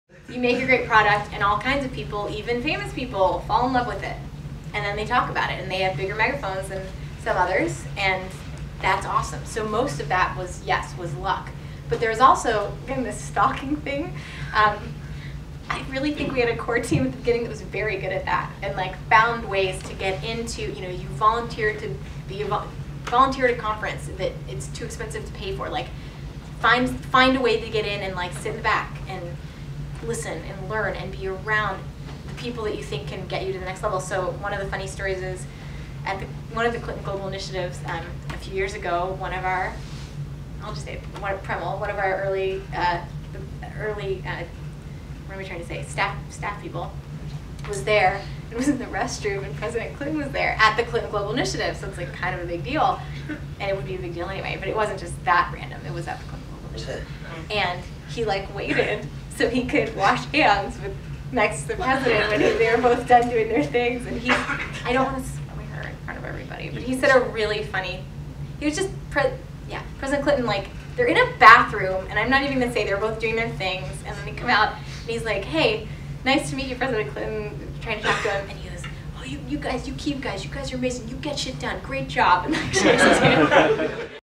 President Clinton Loves Kiva (Jessica Jackley)
Visionary co-founder of Kiva, the most successful person-to-person micro-lending website in the world.